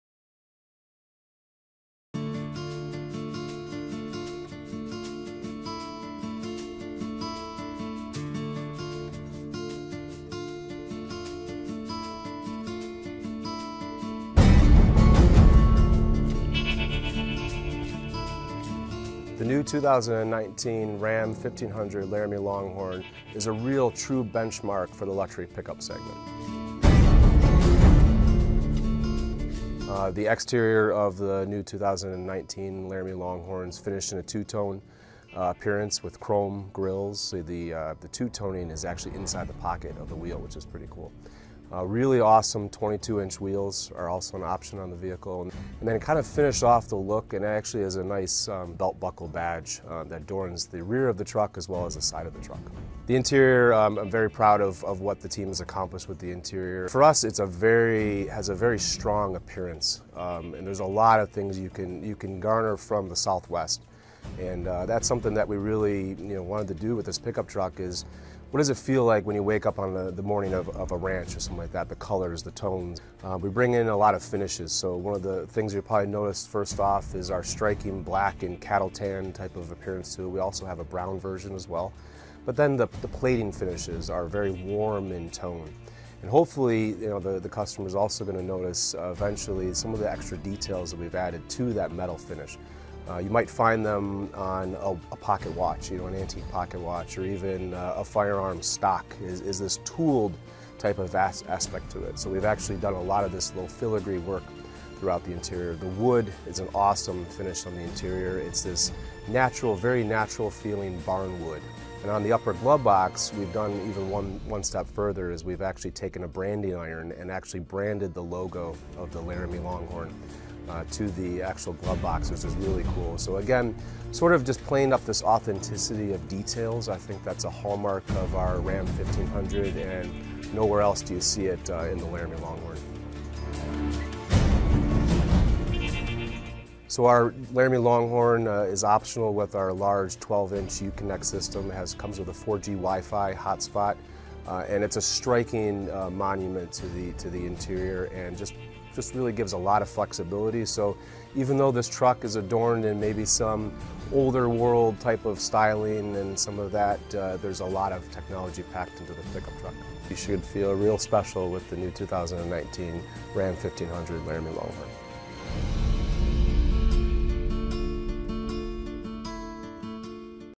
2019 Ram 1500 Laramie Longhorn Edition / Features
Ram announces the all-new 2019 Ram 1500 Laramie Longhorn Edition — the no compromise luxury pickup leading in style, durability and efficiency.

The Ram Laramie Longhorn is the world’s benchmark for upscale pickup trucks, earning the title Luxury Pickup Truck of Texas from the Texas Auto Writers Association (TAWA) two years in a row.

“The all-new 2019 Ram 1500 Laramie Longhorn is a no-compromise luxury truck that delivers what buyers want most: strength, durability, technology and efficiency,” said Mike Manley, head of Ram brand. “Wherever you look, whatever you touch, the Laramie Longhorn is filled with more craftsman-like details, including wood surfaces and 100 percent full-grain leather seats, than any other truck in the segment.”

Layered with premium materials, the Laramie Longhorn carries iconic features and design cues from traditionally handcrafted, time-tested wares such as the antique pocket watch, hand-tooled leather cowboy boots or a horseman’s saddle. The Ram Laramie Longhorn’s wood, metal and leather elements are all authentic.

Ram 1500 Laramie Longhorn models are distinguished externally by the standard two-tone RV Match Brown trim or monotone paint, premium LED headlamps and tail lamps, and unique wheels and grilles.

Inside, the 2019 Ram 1500’s Laramie Longhorn trim exudes quality with a segment-exclusive hand-wrapped, 100 percent full-grain leather dashboard and southwestern-design-inspired door-panel and armrest coverings. Throughout the cabin, a new embossed alligator skin pattern leather covers surfaces including the center console, instrument panel, seats and door panel inserts. Other unique details include satin chrome accents, a brushed-nickel metal badge flush-mounted on the console lid and a new “Longhorn” branding mark burned on the wood-trimmed upper glove box door.

Real barn-wood accents are used throughout the cabin. Wood tandem doors on the center console reflect the heightened attention to detail and provide closed storage. At night, the cabin is accentuated with ambient lighting to create a luxurious and soothing atmosphere. Longhorn interior colors include Mountain Brown/Light Mountain Brown and Black/Cattle Tan full-leather seats with contrast piping. Heated, full-leather rear seats and class-exclusive rear seat ventilation are available on Longhorn.

A chrome billet “RAM” grille and chrome header surround the premium LED lamps with Adaptive Front-lighting System (AFS). Chrome bumpers, tow hooks and side steps come standard as do wheel flares. Two-tone color options complement the RV Match Walnut Brown paint. New 20- or  22-inch wheels are offered with a variety of complementary finishes. The Laramie Longhorn Edition belt-buckle badge appears on the front doors and tailgate.

Ram's new 1500 is the benchmark for technology and connectivity in pickups offering 4G WiFi hot spot, exclusive SiriusXM 360L personalized listening experience, SiriusXM Guardian, five USB ports to link with the award-winning Uconnect system and a segment-leading 12-inch touchscreen featuring split-screen capability for dual-application operation or full-screen single application operation.

The new 2019 Ram 1500 offers truck buyers leading-edge innovation with 225 pounds of weight reduction, up to 12,750 pounds of towing capability, the eTorque mild hybrid system and active systems that improve fuel efficiency and assist drivers. The Ram 1500 stands out as the benchmark in a very competitive segment.

subscription  You will receive the latest automotive News.
GOOD LUCK